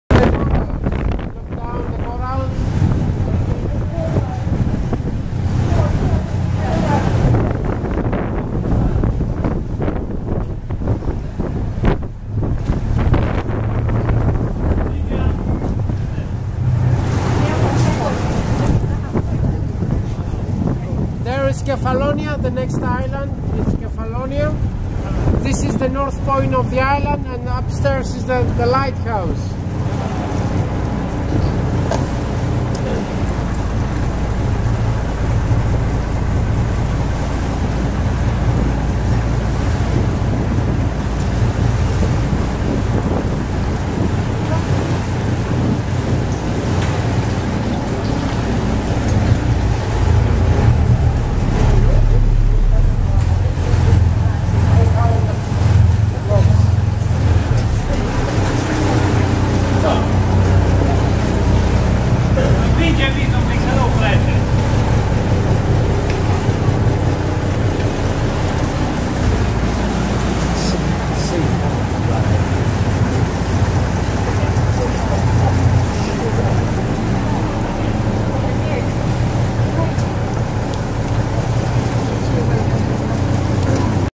Blue Caves, Zakynthos Island, Greece
Niste imagini de neuitat de la Blue Caves, de pe Insula Zakynthos din Grecia :)
Culoarea apei face totul :)
:: Web & IT Solutions Provider]
[Made with Nikon P510]